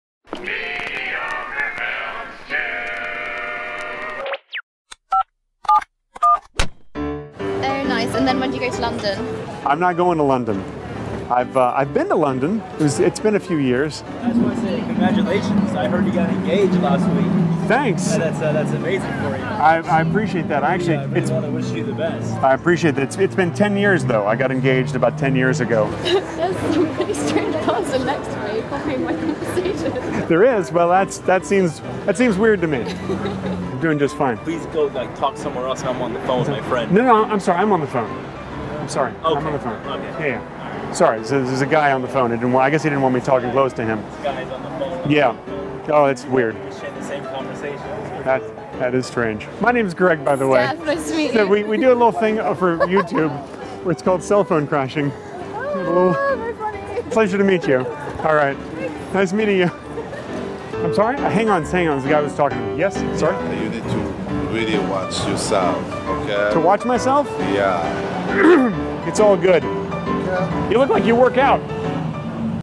MORE Cell Phone Crashing in BEVERLY HILLS!
Don't you hate when people talk loudly on their phones in public?  Why not have some fun with it and "crash" their calls!

Greg